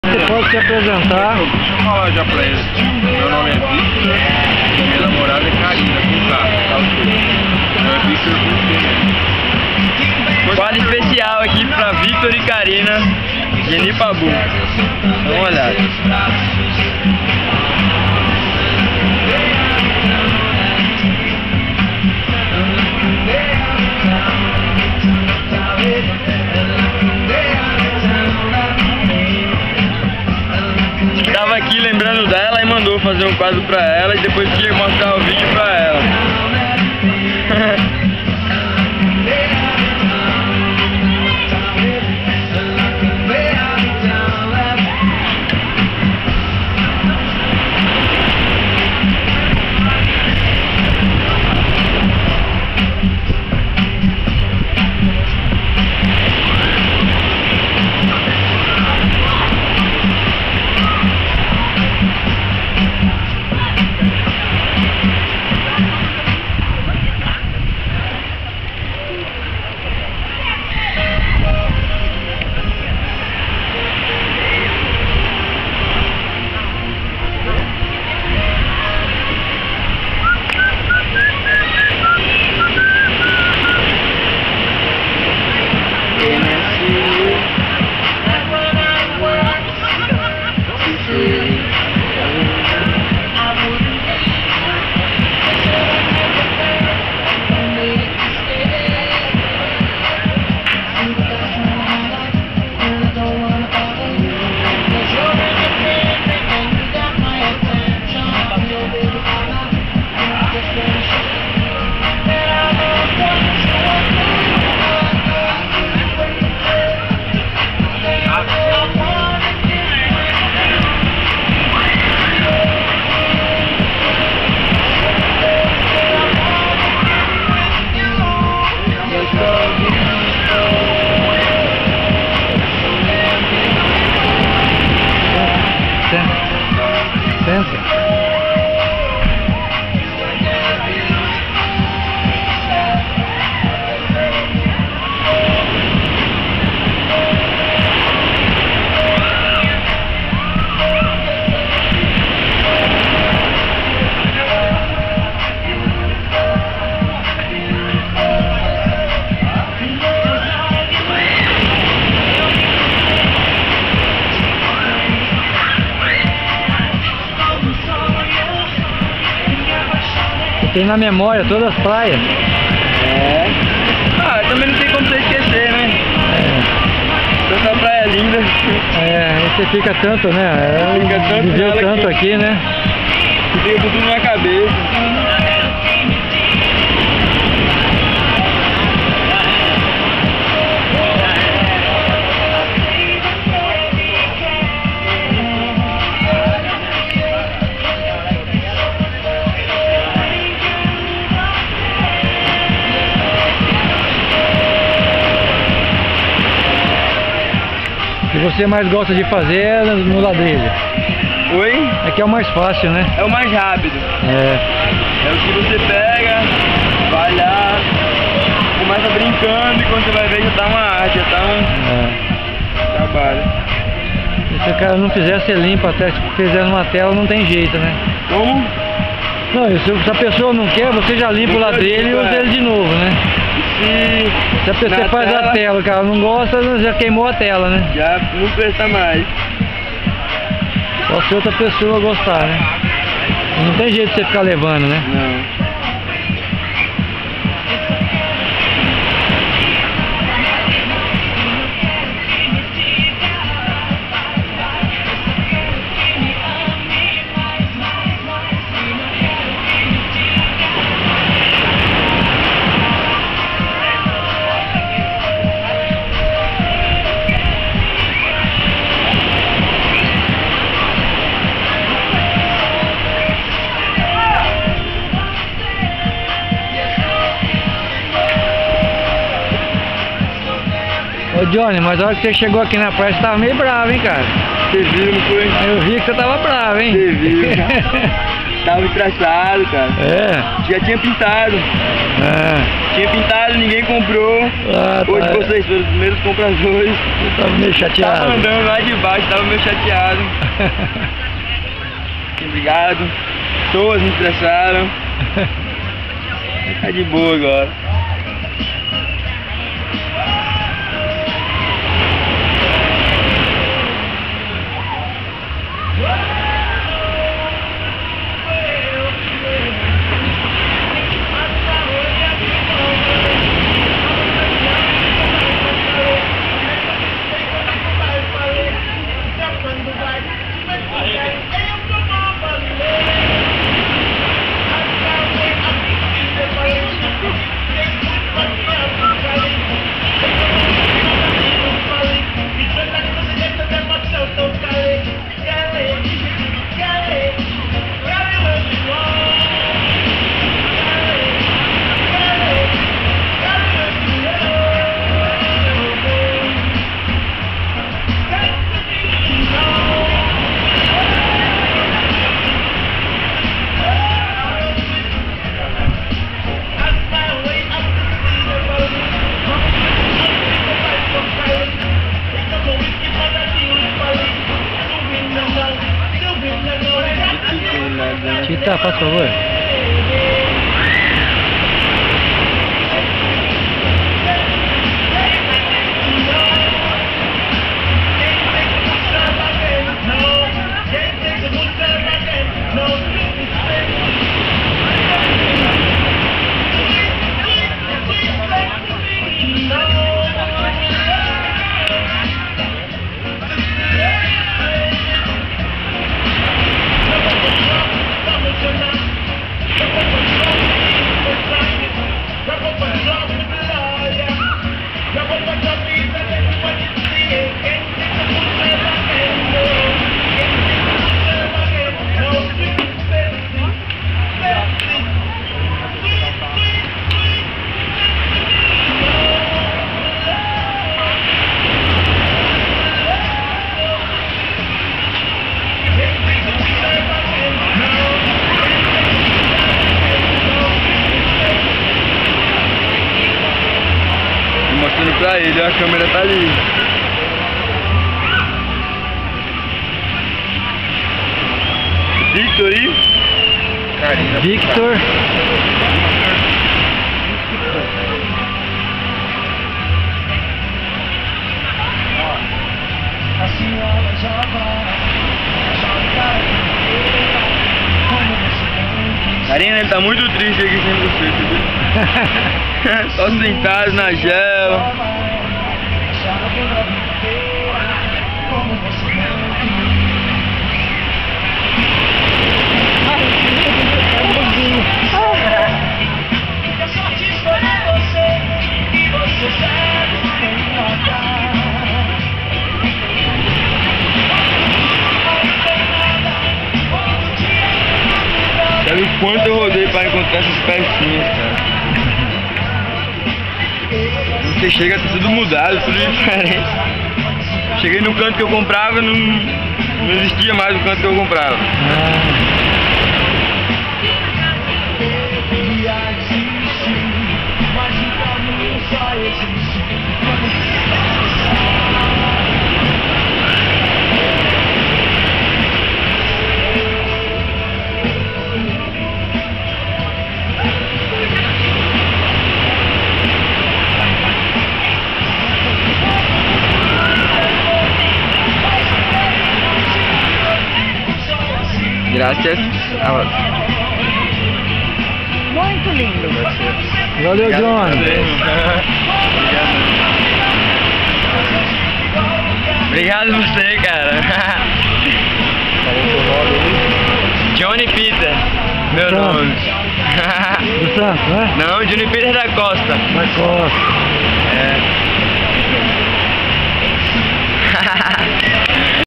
Arte em azulejo
Arte em azulejo para Victor e Karina, feita por Johnny Petter, grande artista do litoral nordestino.